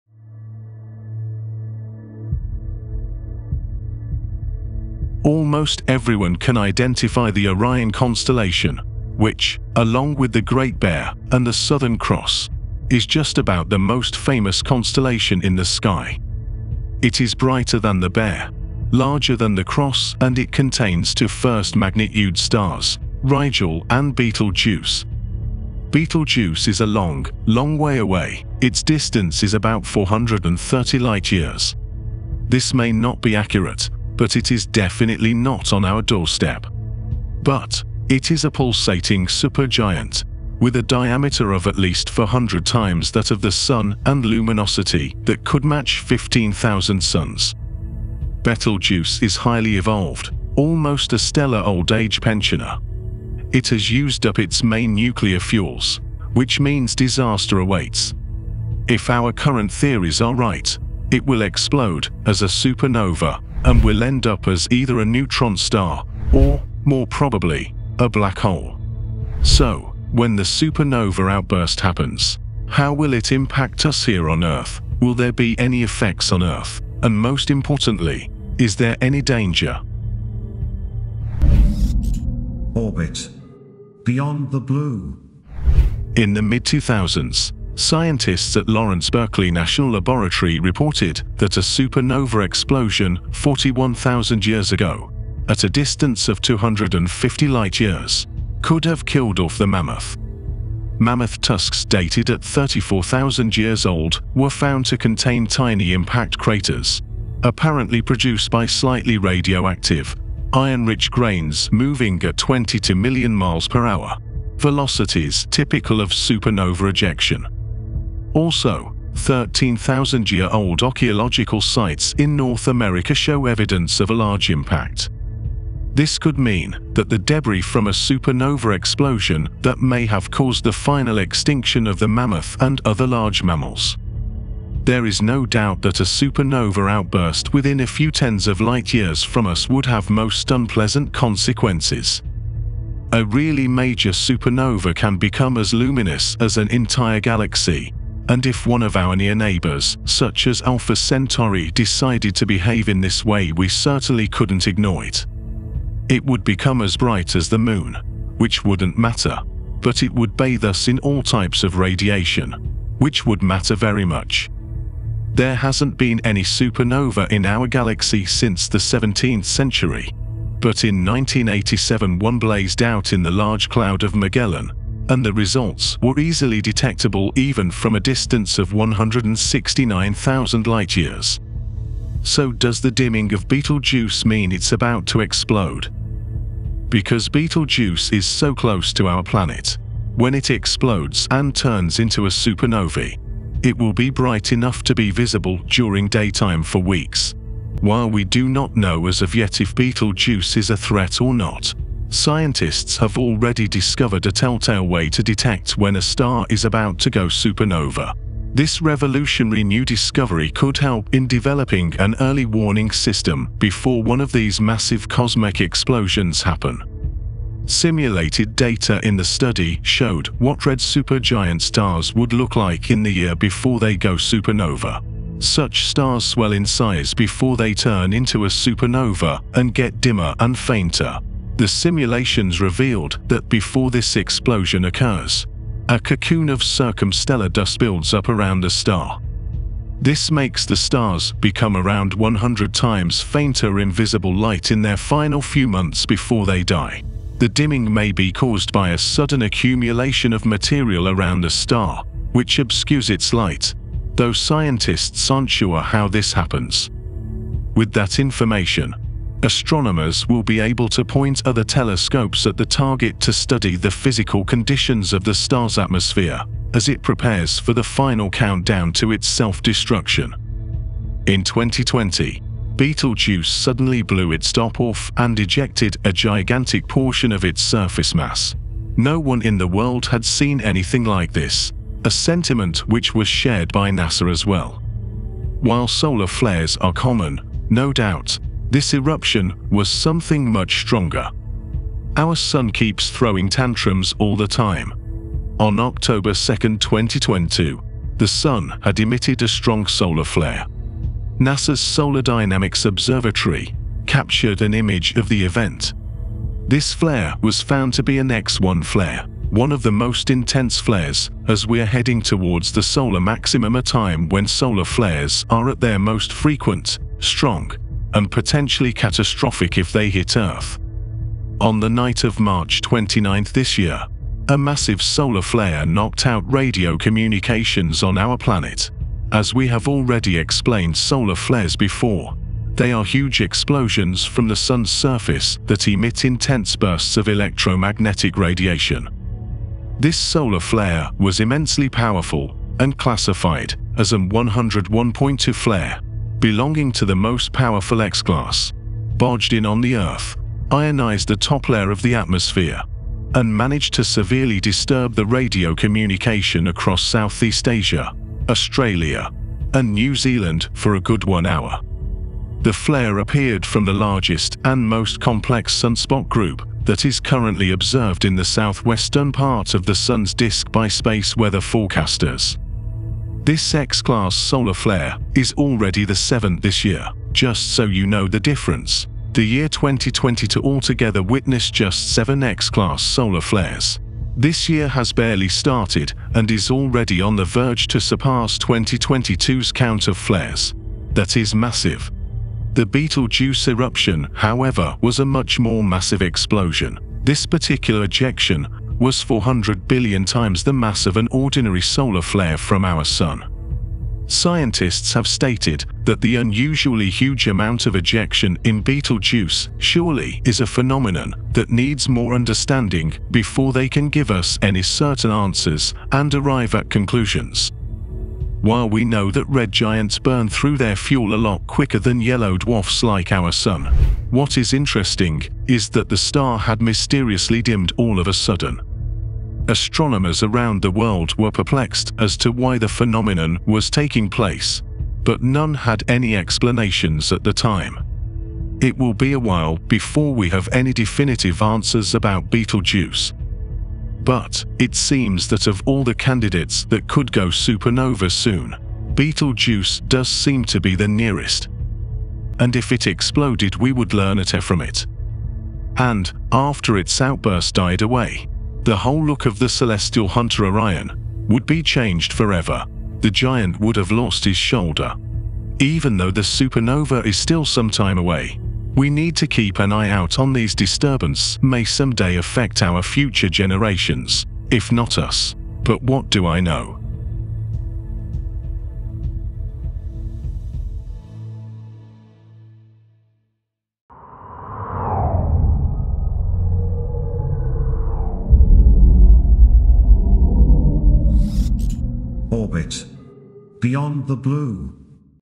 WORRYING New Discovery About Betelgeuse Supernova Explosion!
Worrying New Discovery About Betelgeuse Supernova Explosion in 2023!
Betelgeuse  is a pulsating supergiant, with a diameter of at least 400 times that of the Sun and luminosity that could match 15,000 Suns. It is highly evolved.
It has used up its main nuclear fuels, which means disaster awaits.
If our current theories are right, it will explode as a supernova and will end up as either a neutron star or, more probably, a black hole.
So, when the supernova outburst happens, how will it impact us here on Earth? Will there be any effects on Earth? And most importantly, is there any danger?